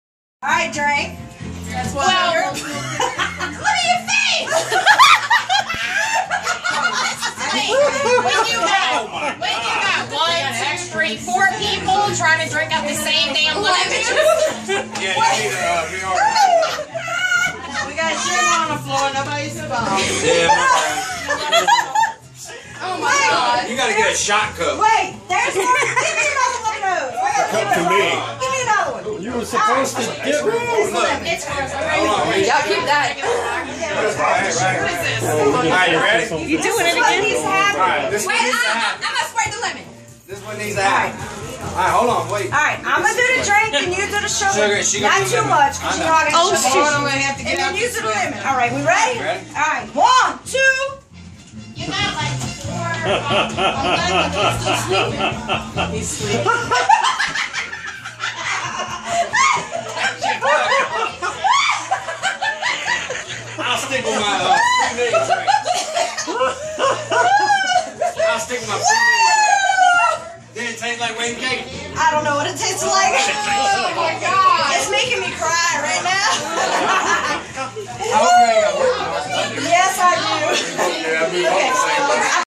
Nastiest shot ever!
We try a new concoction...with cake vodka.